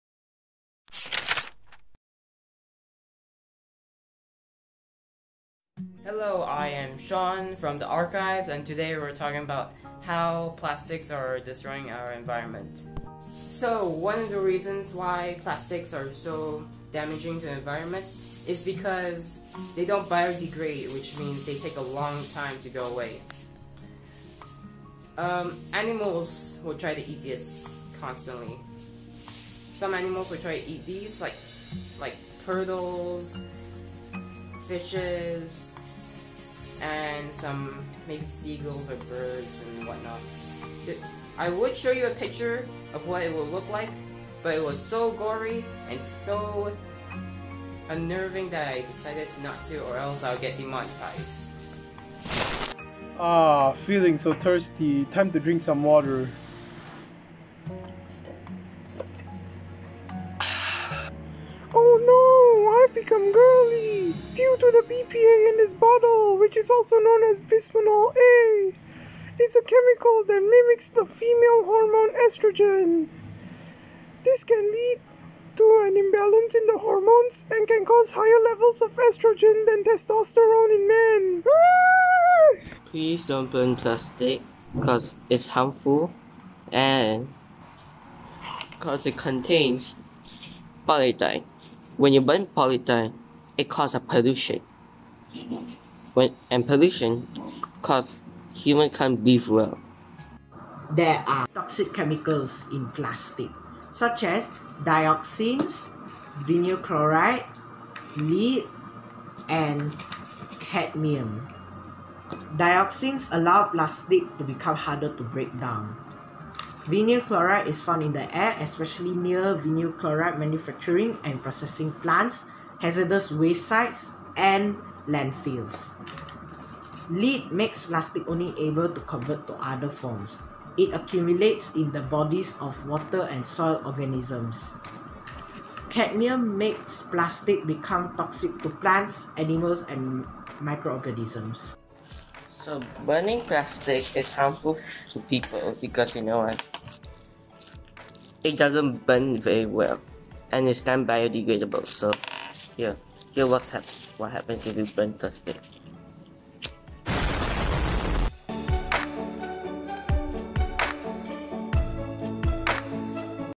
Archives Production (CME Attempt 01)
Thanks to our latest corporate sponsor CM-Eco for this new assignment on Recycling Awareness Campaign.

Our kids at Ai Talent have been very productive. This is their 3rd Job Experiencing Home-based corporate online project since MCO began.

Kids do have amazing learning adaptability when the assignments are immediately relevant to their career.